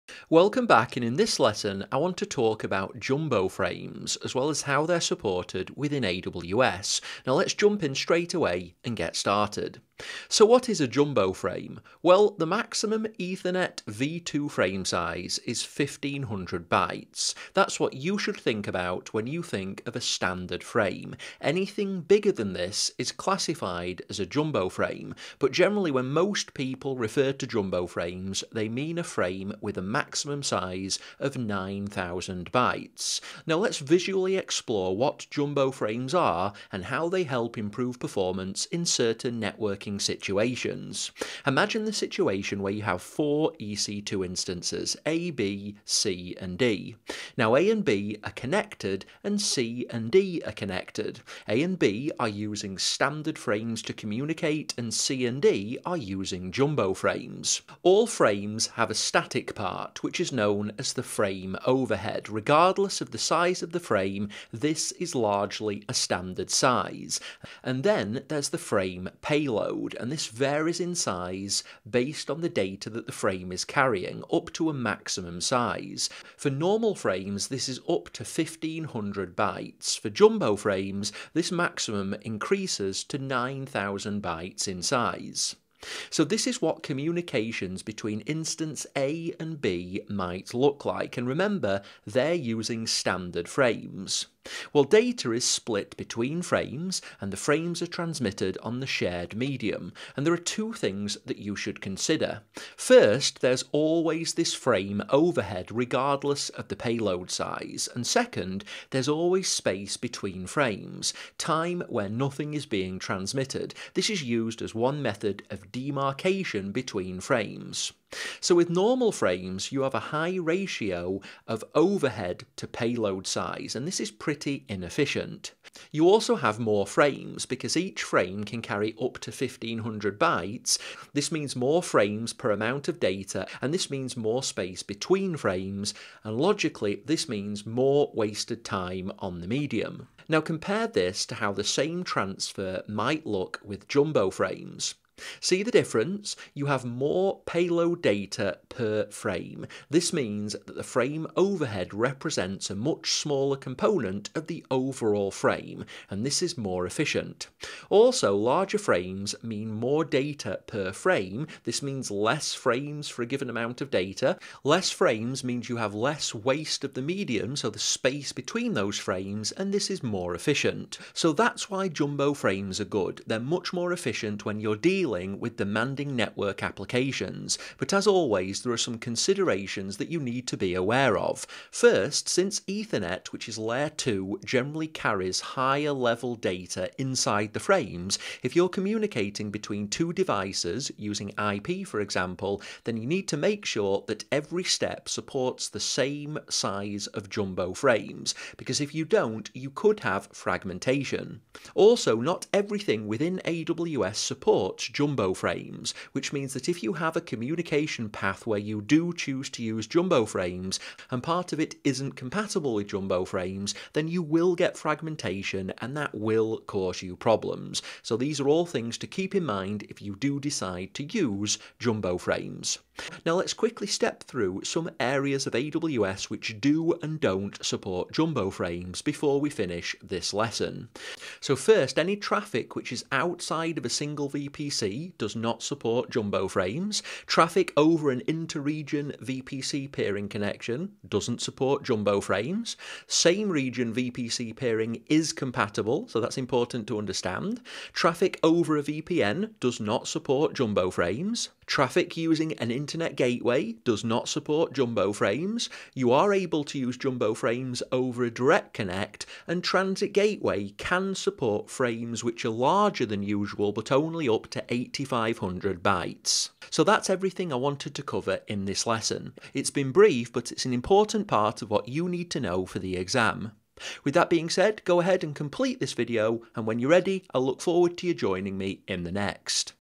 Jumbo Frames and MTU
In this video Adrian explains Jumbo frames and how they work in AWS.

The maximum transmission unit (MTU) of a network connection is the size, in bytes, of the largest permissible packet that can be passed over the connection. The larger the MTU of a connection, the more data that can be passed in a single packet. Ethernet packets consist of the frame, or the actual data you are sending, and the network overhead information that surrounds it.

Ethernet frames can come in different formats, and the most common format is the standard Ethernet v2 frame format. It supports 1500 MTU, which is the largest Ethernet packet size supported over most of the internet. The maximum supported MTU for an instance depends on its instance type. All Amazon EC2 instance types support 1500 MTU, and many current instance sizes support 9001 MTU, or jumbo frames.